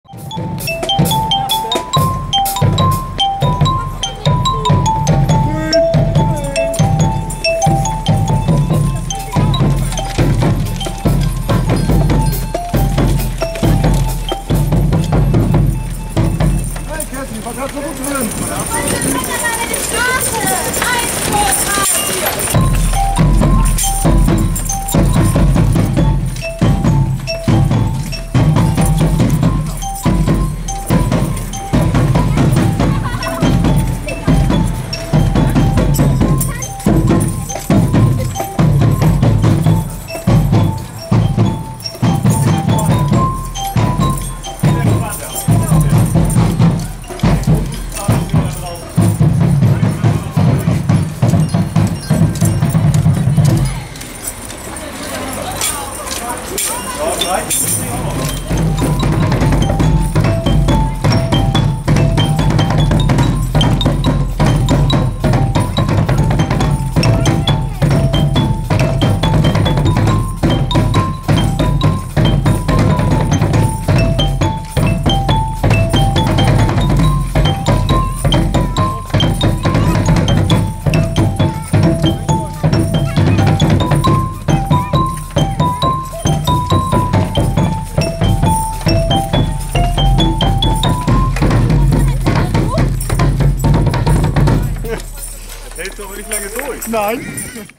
Tag der Küstenwache, nach der Veranstaltung 2013
Lübecks Freibeutermukke beim Tag der Küstenwache in Neustadt in Holstein, 17. August 2013. Marsch zum Parkplatz. Natürlich mit Musik.

Mehr Mukke:
freibeutermukke.eu